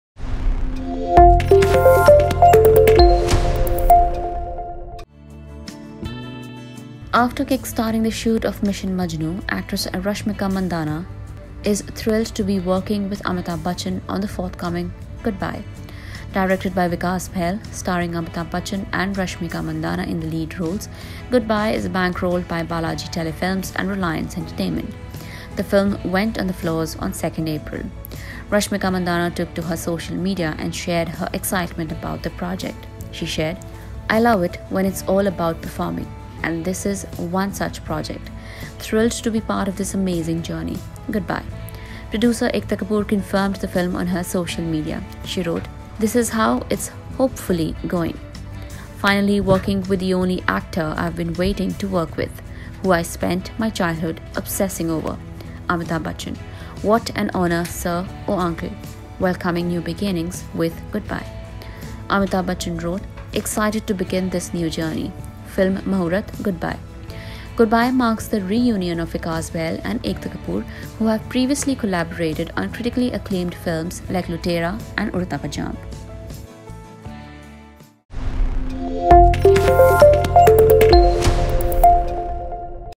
Thrilled To Be Part Of Goodbye: Rashmika Mandanna
After kick-starting the shoot of Mission Majnu, actress Rashmika Mandanna is thrilled to be working with Amitabh Bachchan on the forthcoming Goodbye.

Directed by Vikas Bahl, starring Amitabh Bachchan and Rashmika Mandanna in the lead roles, 'Goodbye' is bankrolled by Balaji Telefilms and Reliance Entertainment. The film went to the floors on 2nd April.

Rashmika Mandanna took to her social media and shared her excitement about the project. She shared,

“I love it when its all about performing...and this is one such project. Thrilled to be a part of this amazing journey...Goodbye!!”

Producer Ekta Kapoor confirmed the film on her social media. She wrote:

“This is how it’s (hopefully) going! Finally working with the ONLY actor I’ve been waiting to work with ... who I spent my childhood obsessing over @amitabhbachchan! Wat an honour sir/uncle! Welcoming new beginnings with Goodbye ...”

Amitabh Bachchan wrote:

"T 3862 -Excited to begin this new journey!

Film Mahurat Goodbye..."

Goodbye marks the reunion of Vikas Bahl and Ekta Kapoor, who have previously collaborated on critically acclaimed films like Lootera and Udta Punjab.